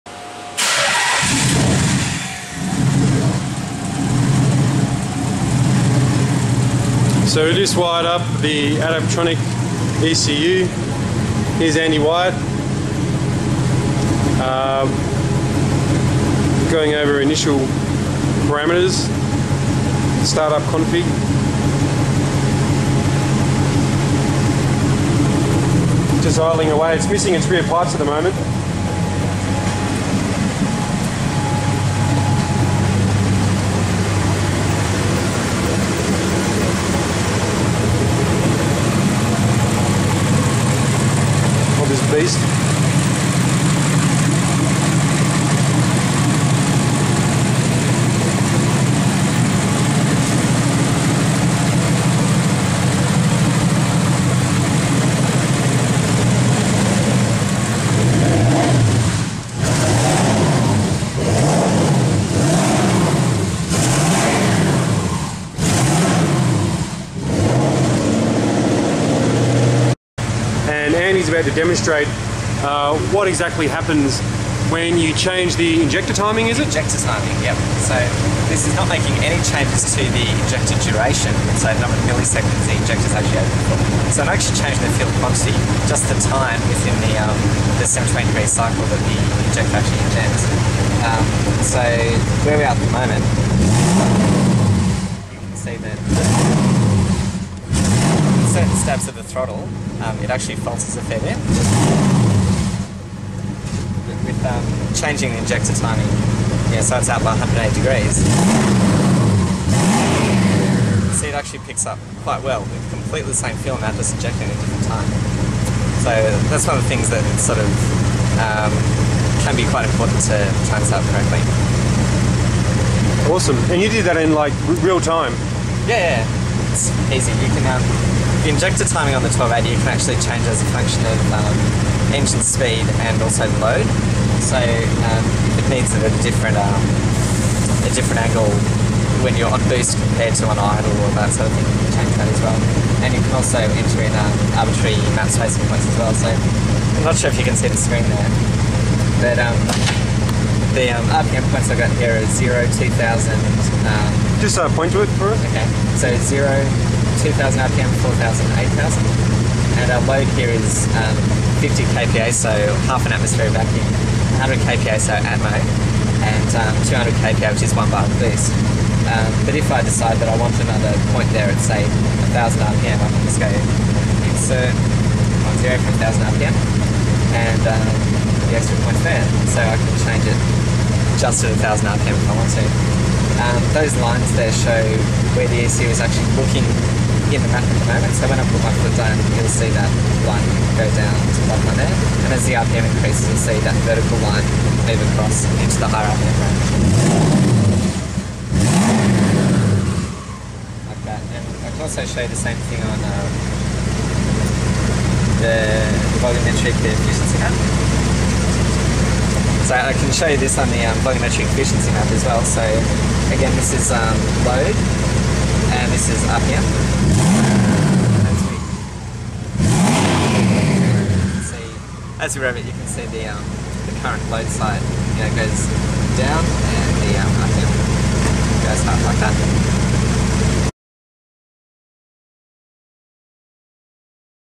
Zoom Magazine -- Adaptronic ECU Workshop visit
Zoom Production Editor Glen Fuller visits Adaptronic where Andy Wyatt is installing a 1280 ECU to the twin-turbo VH45-powered R31 skyline coupe.